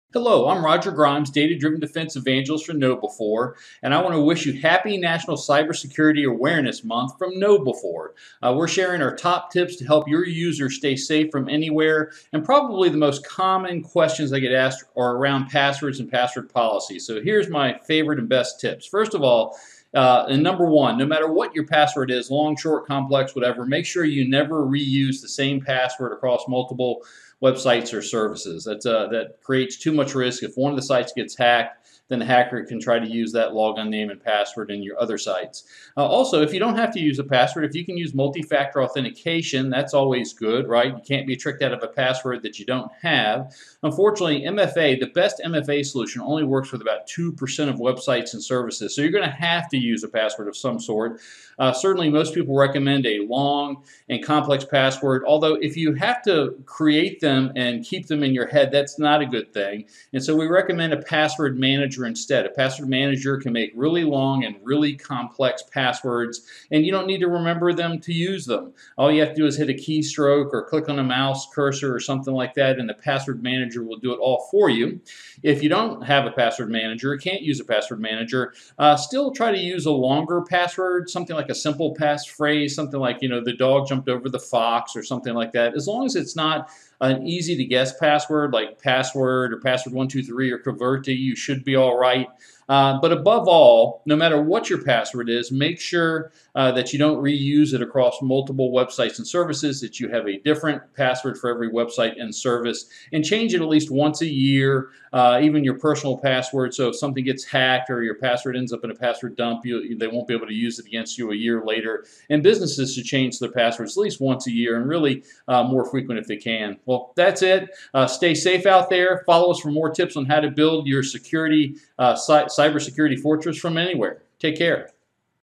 Cybersecurity Awareness Month Weekly Tip: Password Security
Roger A. Grimes, KnowBe4's Data-Driven Defense Evangelist, addresses how users can ensure their passwords are secure as well as effective password policies you can implement within your organization.

- Learn more about KnowBe4's integrated Security Awareness Training and Simulated Phishing platform